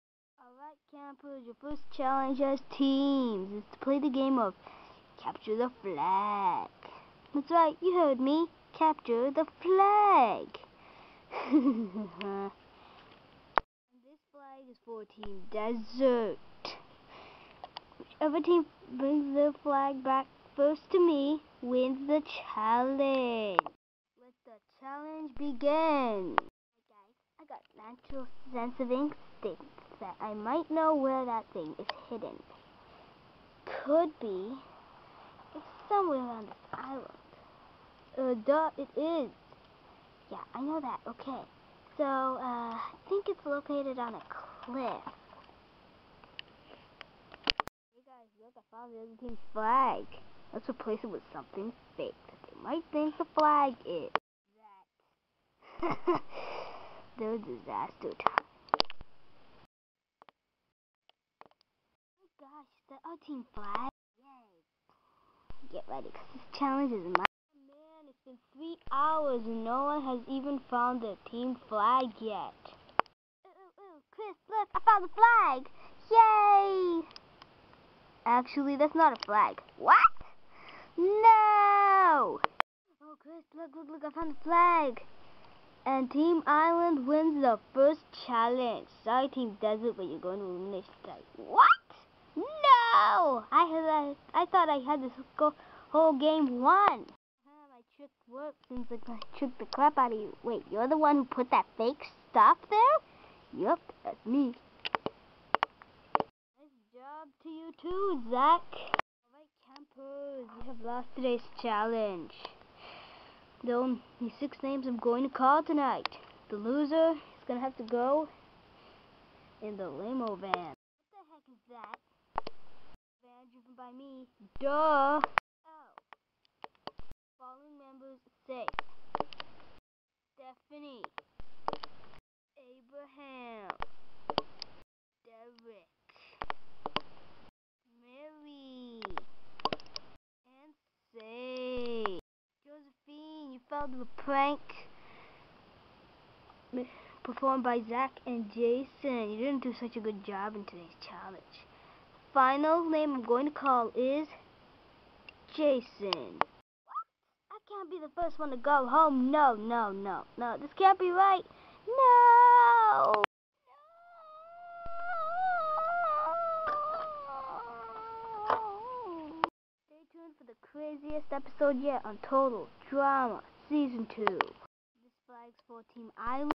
total drama lego season 2 episode 1
This is the first episode of season 2,who will leave next?find out here on total drama lego season 2!